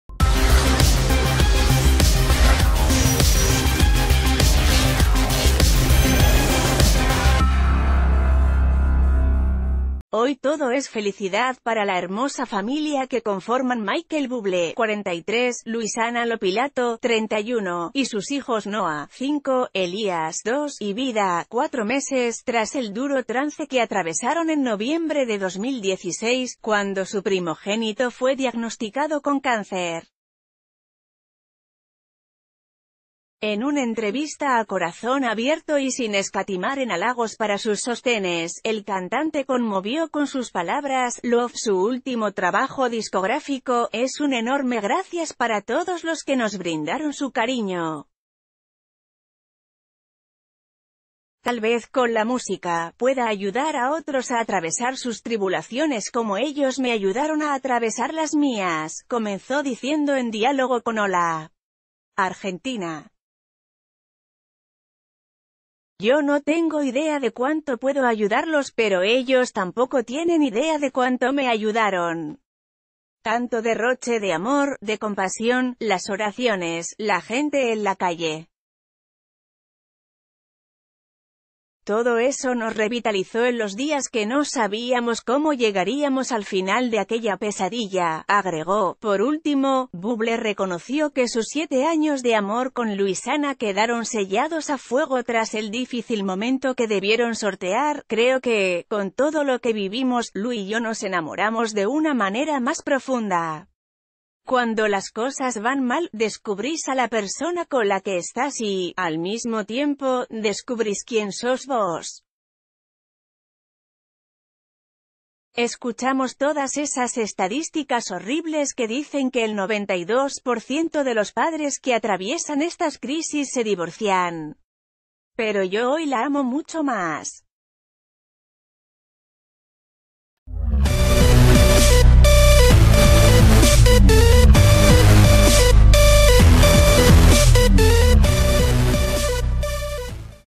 La emotiva confesión de Michael Bublé: "Con todo lo que vivimos con nuestro hijo..."
Hoy todo es felicidad para la hermosa familia que conforman Michael Bublé (43), Luisana Lopilato (31) y sus hijos Noah (5), Elías (2) y Vida (4 meses) tras el duro trance que atravesaron en noviembre de 2016, cuando su primogénito fue diagnosticado con cáncer. En un entrevista a corazón abierto y si...